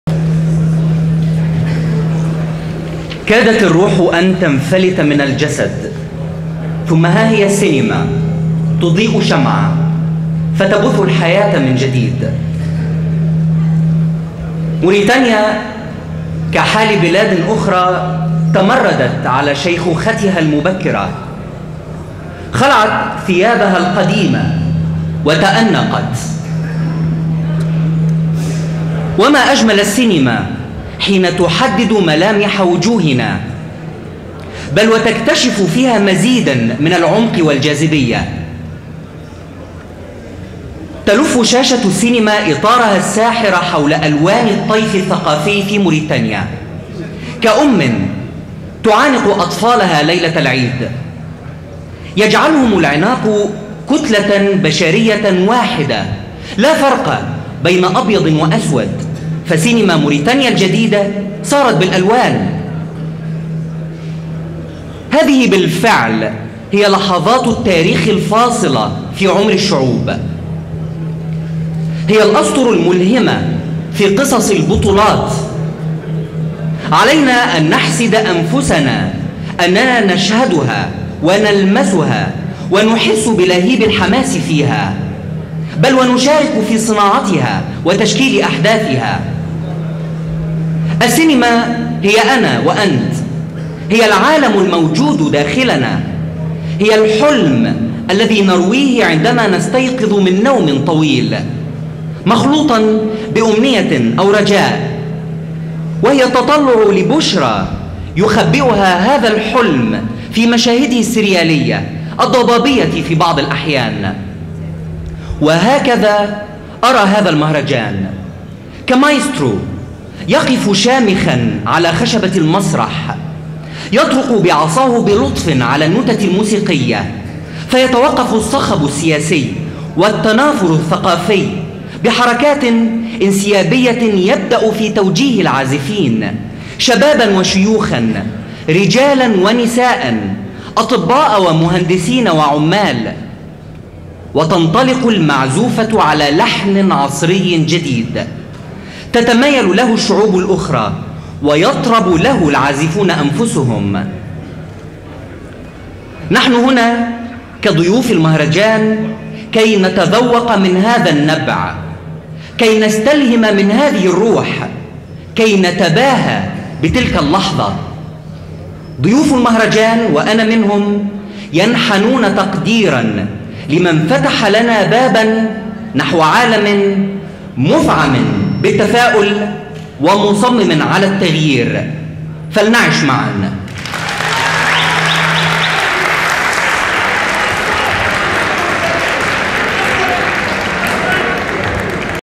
كلمة محمد سعيد محفوظ في مهرجان موريتانيا 2009
كلمة ضيوف المهرجان في حفل افتتاح الدورة الرابعة من أسبوع الفيلم الموريتاني في نواكشوط من 23 إلى 29 أكتوبر 2009، يلقيها نيابة عنهم محمد سعيد محفوظ، عضو لجنة التحكيم في المهرجان.